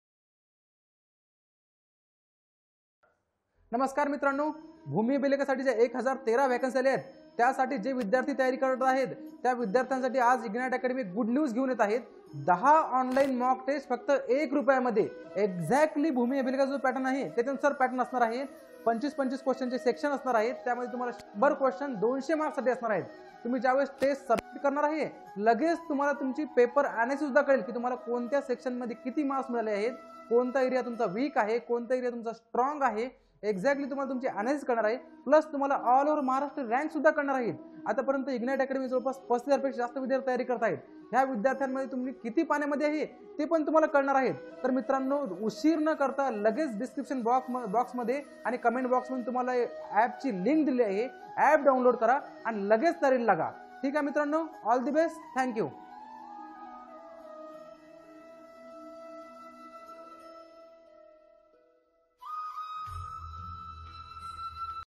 भूमीअभिलेख 10 ऑनलाइन सराव पेपर्स फक्त 1 रु. मध्ये | BhumiAbhilekha 10 Online Mock Test For 1 Rs.
भूमीअभिलेख 10 ऑनलाइन सराव पेपर्स फक्त 1 रु. मध्ये
BhumiAbhilekha 10 Online Mock Test For 1 Rs.

Pattern Exactly As Given By Bhumiabhilekha
100 Question For 200 Marks

Identify Your Weak & Strong Areas Of Exam